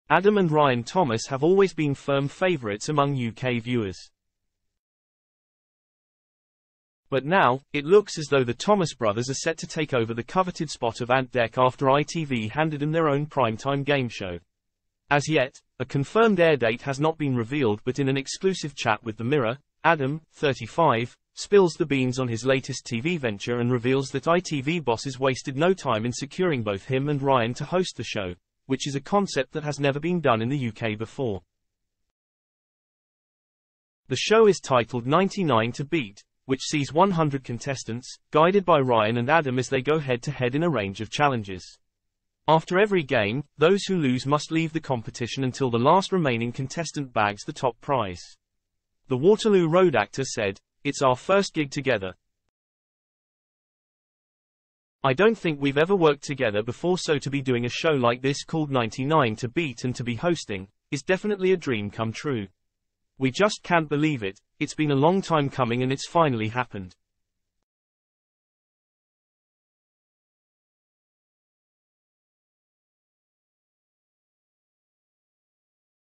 Adam Thomas confirms ITV bosses headhunted him and brother Ryan to be new Ant & Dec with huge games
Adam and Ryan Thomas have always been firm favourites among UK viewers.

But now, it looks as though the Thomas brothers are set to take over the coveted spot of Ant & Dec after ITV handed them their own prime-time gameshow. As yet, a confirmed air date has not been revealed but in an exclusive chat with the Mirror, Adam, 35, spills the beans on his latest TV venture and reveals that ITV bosses wasted no time in securing both him and Ryan to host the show, which is a concept that has never been done in the UK before.

The show is titled 99 To Beat, which sees 100 contestants, guided by Ryan and Adam as they go head to head in a range of challenges. After every game, those who lose must leave the competition until the last remaining contestant bags the top prize. The Waterloo Road actor said: "It's our first gig together. I don't think we've ever worked together before so to be doing a show like this called 99 To Beat and to be hosting, is definitely a dream come true. We just can't believe it, it's been a long time coming and it's finally happened."